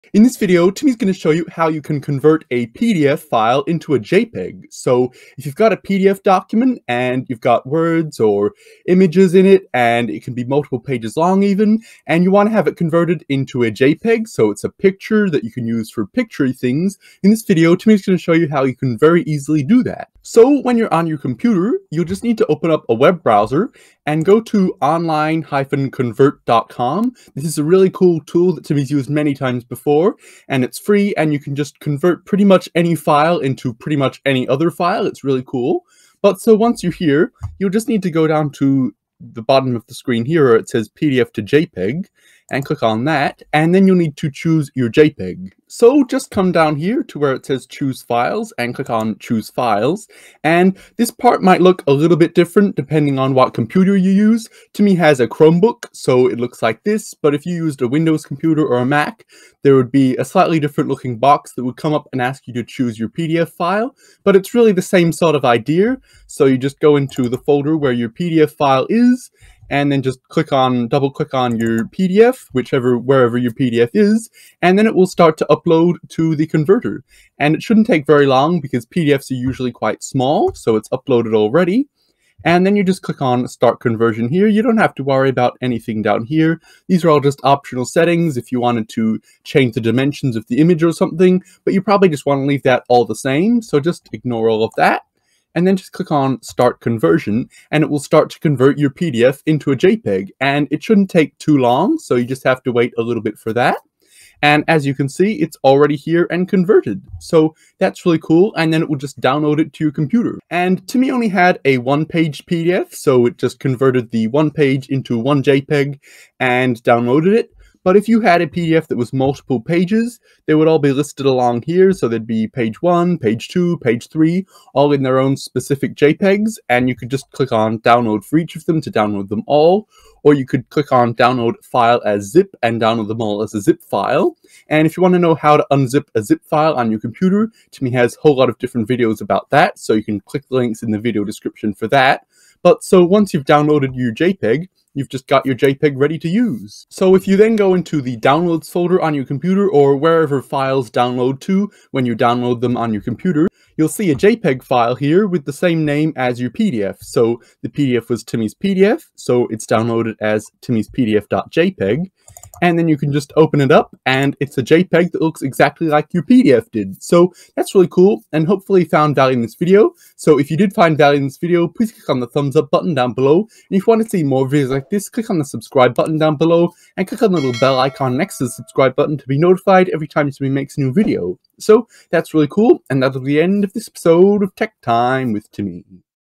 How To Convert A PDF To A jpg
In this video, Timmy shows you how to convert a pdf into a jpeg using a free simple online tool!

If you have a pdf that you need to convert into a jpeg, it may seem like a complicated task, but there's actually a very simple way to do it using a free online tool. Here's a link to it:

And if you convert a pdf with multiple pages and need help extracting the files from the zip file, here are links to my videos on extracting zip files:

If you have an Android phone or tablet, you can follow the steps in this video:

If you have the zip file saved on Google Drive, you can follow the steps in this video:

And Timmy doesn't have a video on how to do it on a Mac, but here's a good video from "macmostvideo":